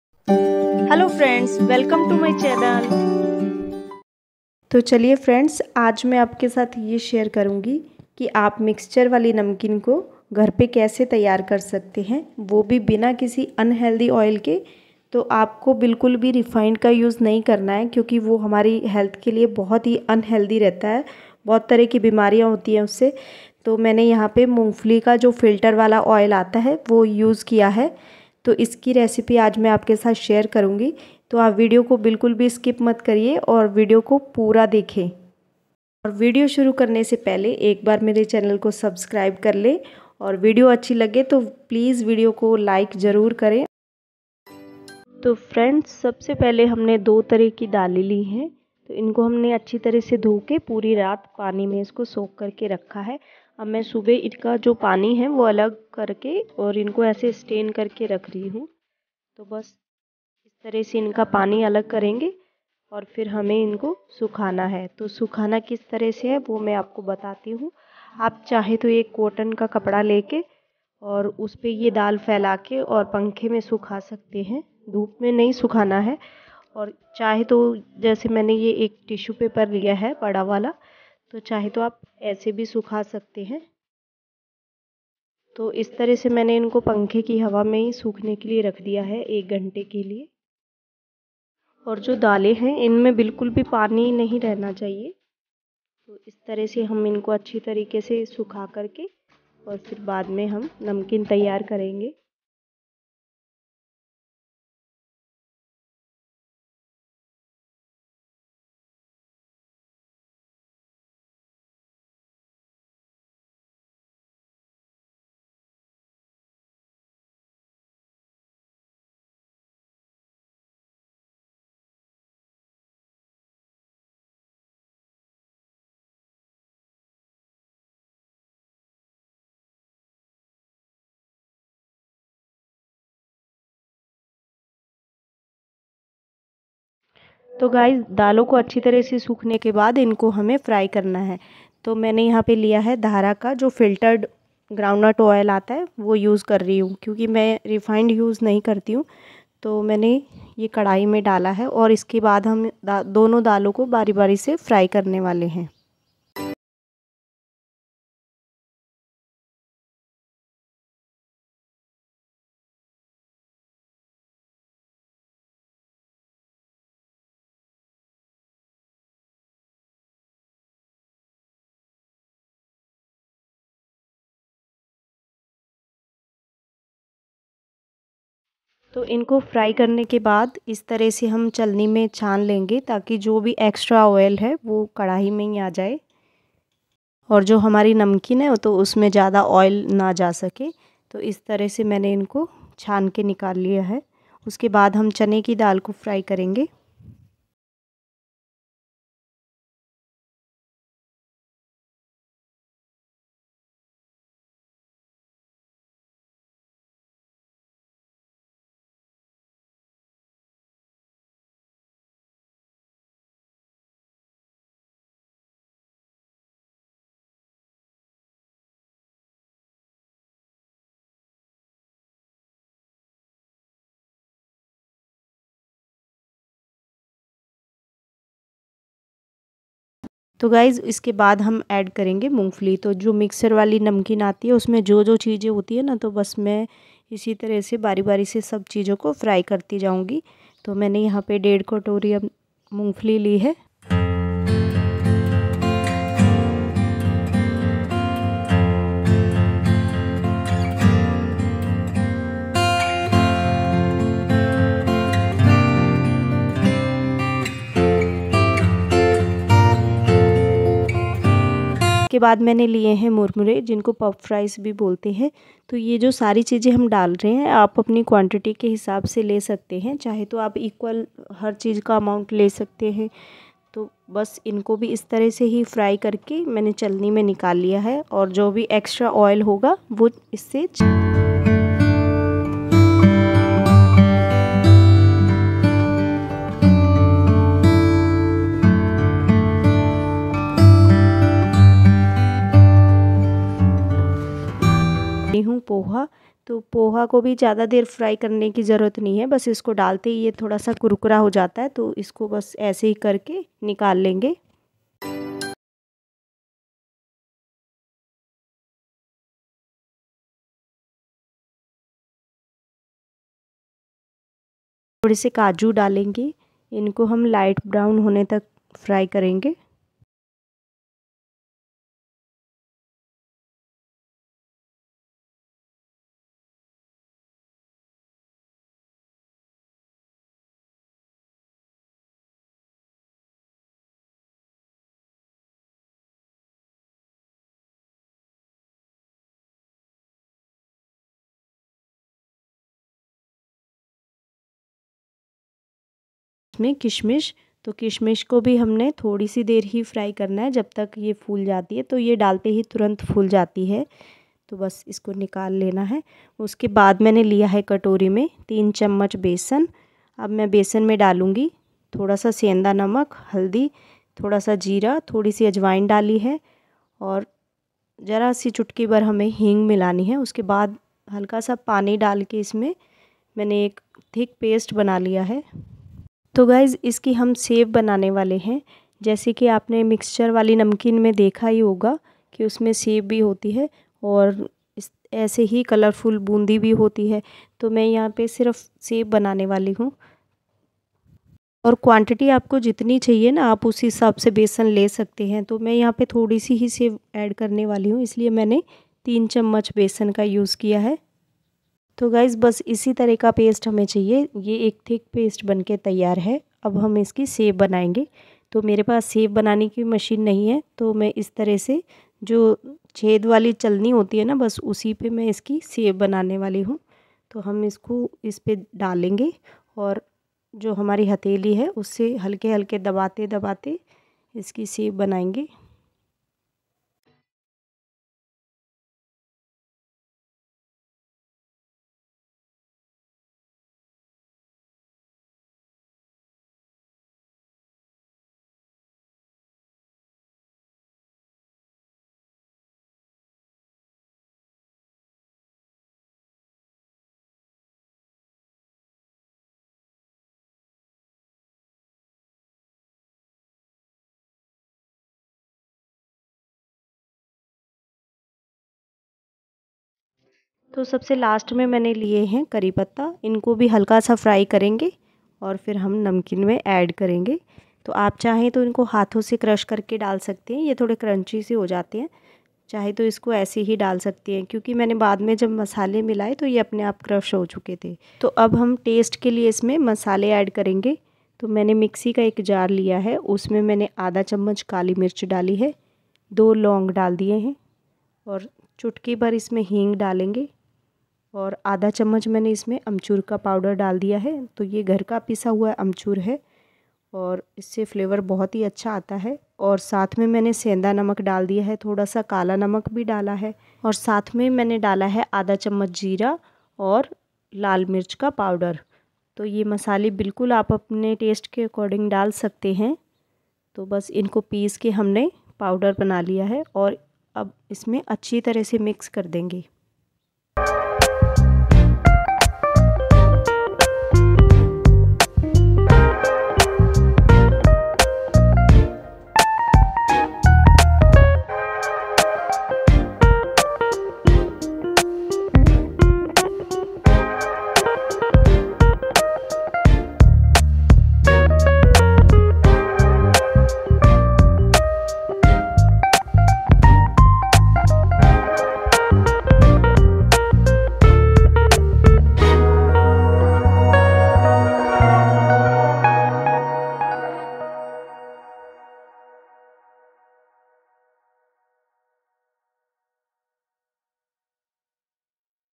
बाज़ार की रिफाइंड तेल वाली नमकीन छोड़ दें। खाएं इन तेलों वाली नमकीन।
healthy cooking oil for namkeen
homemade namkeen
mixture namkeen
snacks for tea time
mixture namkeen kaise banate hain